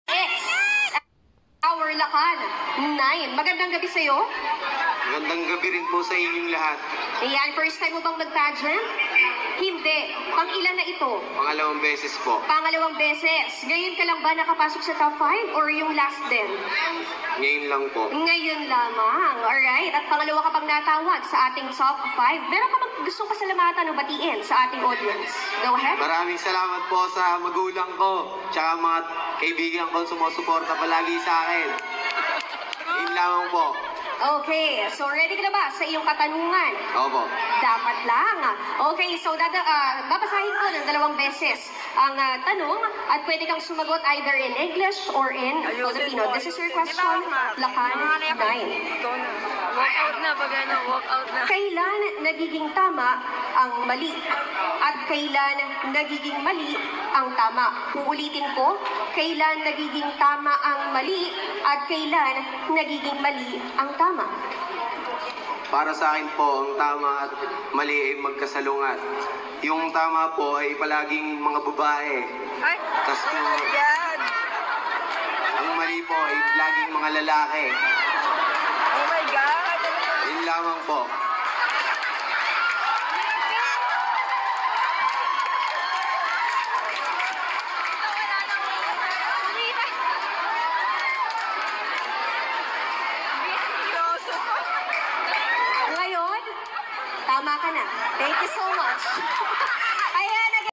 Lakan Question and Answer portion: Kailan nagiging tama ang mali at kailan nagiging mali ang tama
Just for Fun